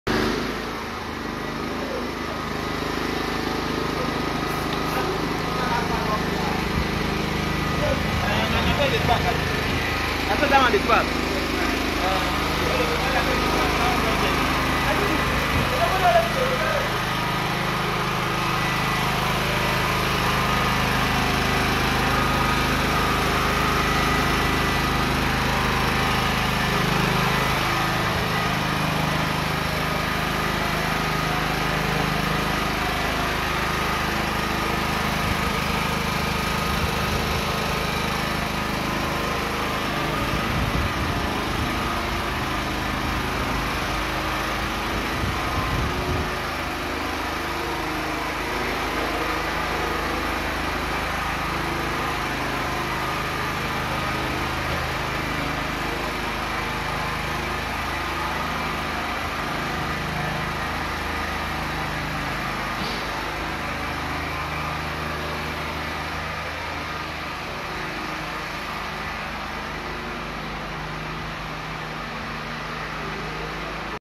Aluminium casement windows with burglary and balcony glass rail
This is a quality Aluminium Windows and balcony glass rail, the window is fabricated with good quality Aluminium profile glazed with 5mm glass complete with burglary proof and net.. contact us for more info @ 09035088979